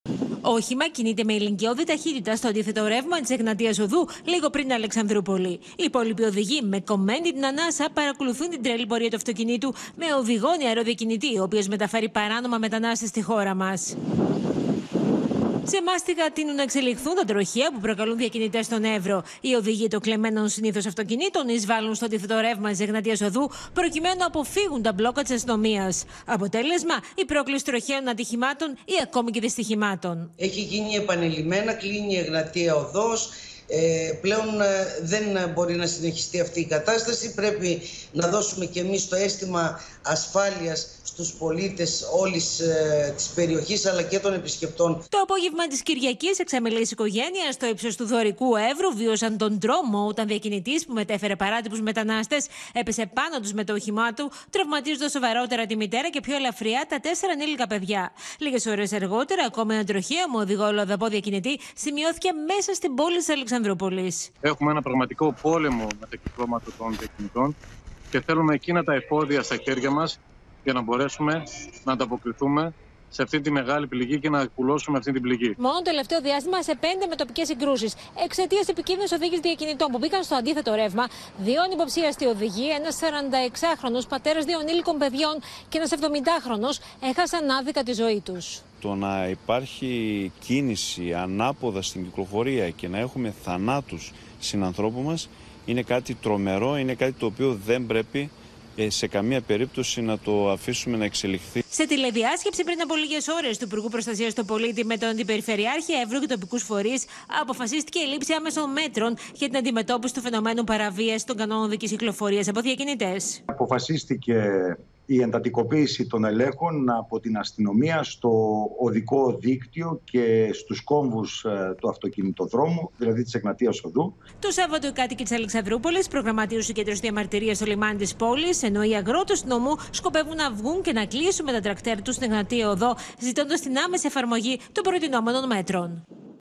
Έβρος: Καθημερινά πλέον τα περιστατικά επικίνδυνης οδήγησης διακινητών μεταναστών | OPEN TV
Γίνετε μέλος στο κανάλι του OPEN για να αποκτήσετε πρόσβαση σε προνόμια:
Ανάστατοι είναι οι κάτοικοι του νομού Έβρου με τα -σχεδόν- καθημερινά περιστατικά ανάποδης οδήγησης στην Εγνατία Οδό από διακινητές που μεταφέρουν παράνομα στη χώρα μας μετανάστες. Εξαμελής οικογένεια κινδύνευσε σοβαρά και μάλιστα τραυματίστηκαν η μητέρα με τα τέσσερα παιδιά, ύστερα από μετωπική σύγκρουση με όχημα που οδηγούσε νεαρός διακινητής. Άλλοι δύο συνάνθρωποι μας έχασαν την ζωή τους το προηγούμενο διάστημα εξαιτίας της επικίνδυνης οδήγησης των διακινητών. Σε κατεπείγουσα σύσκεψη του υπουργού Προστασίας του Πολίτη με τον Αντιπεριφερειάρχη Έβρου, αποφασίστηκε η λήψη άμεσων μέτρων.